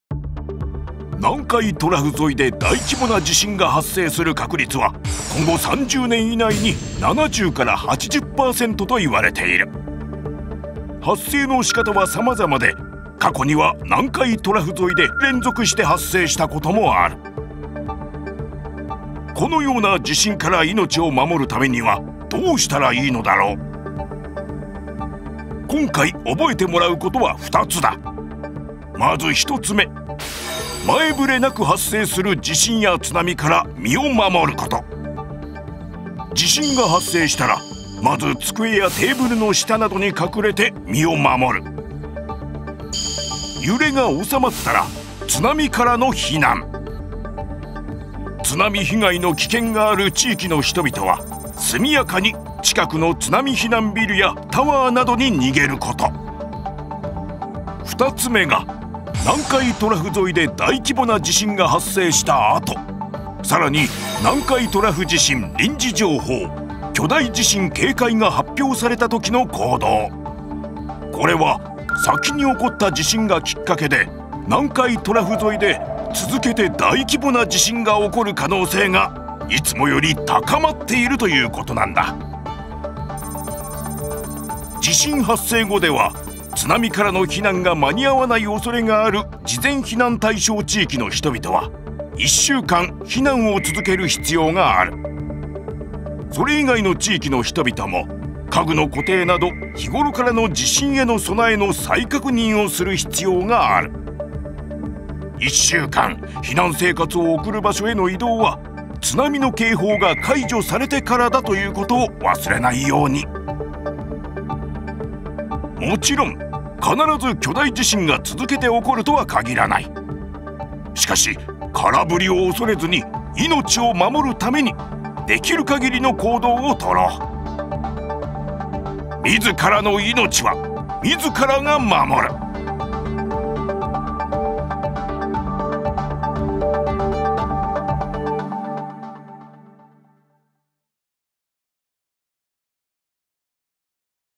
【ダイジェスト版手話付】南海トラフ地震どうなる？どうする？時間差で起こりうる次の地震への備え
この動画は、南海トラフ沿いで地震が発生した後、時間差で巨大地震の発生する可能性が相対的に高まった場合、必要となる防災対応について、ご理解を頂くために作成したものです。
・【完全版手話付】南海トラフ地震どうなる？どうする？時間差で起こりうる次の地震への備え